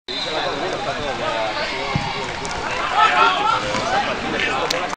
(Archivo) Incursión del Villa. Partido de fútbol UD Villa Santa Brígida-AD Alcorcón. 2ªB. 20/3/2008.
Incursión del Villa. Partido de fútbol UD Villa Santa Brígida-AD Alcorcón. Segunda División B. Grupo I. Jornada 30. 20/3/2008.
Resultado final: 0-3.